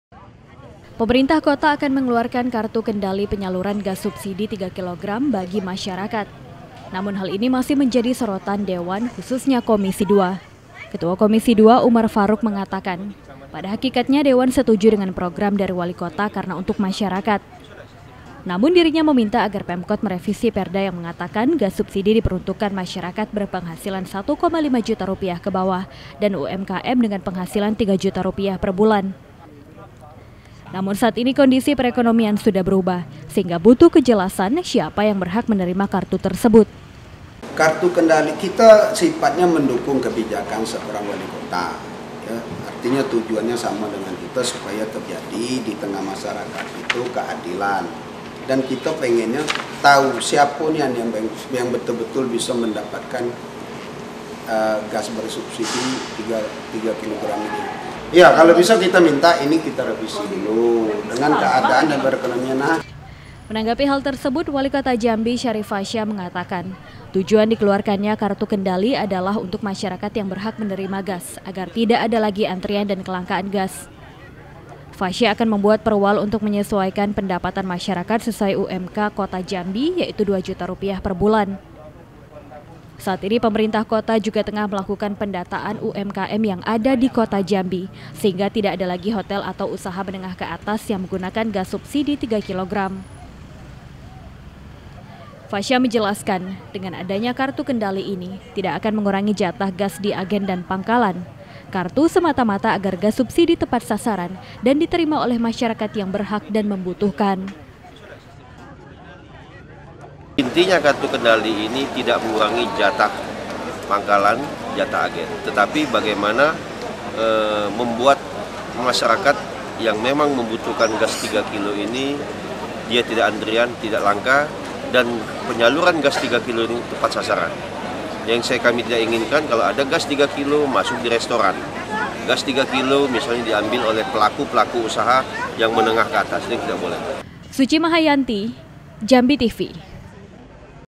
RENCANA KARTU KENDALI GAS 3KG KOMISI II DPRD KOTA MINTA PERDA DIREVISI
RENCANA KARTU KENDALI GAS 3KG
Pemerintah kota akan mengeluarkan kartu kendali penyaluran gas subsidi 3 kg bagi masyarakat . Namun hal ini masih menjadi sorotan dewan khususnya komisi ii . Ketua komisi ii umar faruk mengatakan , pada hakikatnya dewan setuju dengan program dari walikota karena untuk masyarakat . Namun dirinya meminta agara pemkot merevisi perda yang mengatakan gas subsidi diperuntukkan masyarakat berpenghasilan 1,5 juta ke bawah, dan umkm dengan penghasilan 3 juta perbulan. Namun saat ini kondisi perekonomian sudah berubah. Sehingga butuh kejelasan siapa yang berhak menerima kartu tersebut.